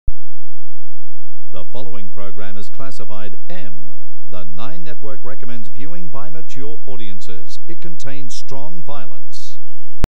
Nine Network Recommends M For Mature Audiences Strong Violence V Warning 2005
Found on a football VHS
The tape was labeled:
"Round 3 NM vs GEE"
but seems to have been recorded over.

Head over to Batprince's game channel:
Youtube.com/Batprince
OR
Head over to Pete's Twitch
Twitch.tv/Peterdea